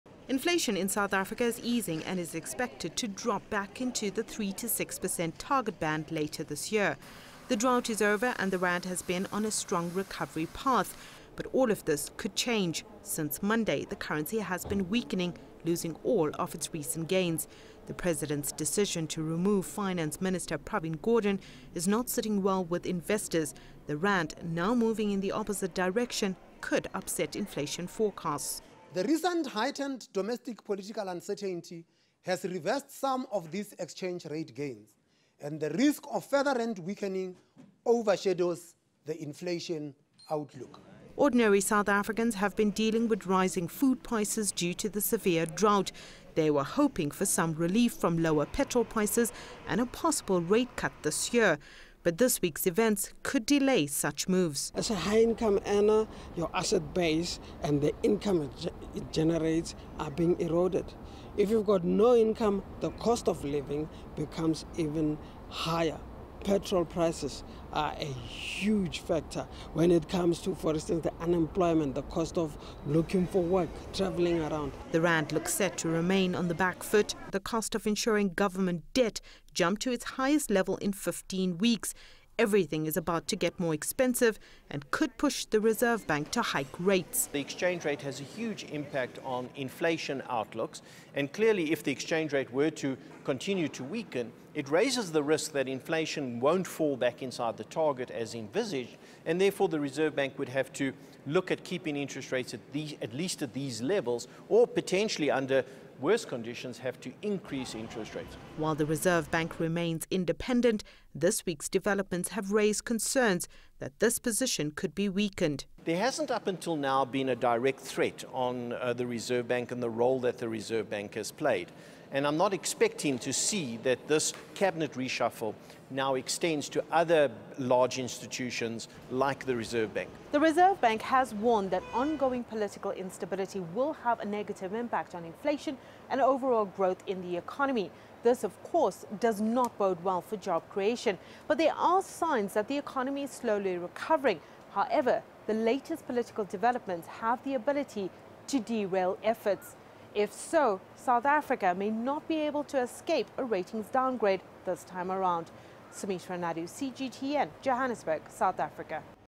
Rand hit a day after Reserve Bank held interest rates at 7%
There is also risk on inflation and interest rates going forward following the late Thursday development in South Africa. While announcing the country's monetary policy decision on Thursday, Reserve bank governor Lesetja Kganyago warned that the positive inflation outlook was at risk. The risk seems to have been realized barely after the words left his mouth. Sumitra Nydoo reports.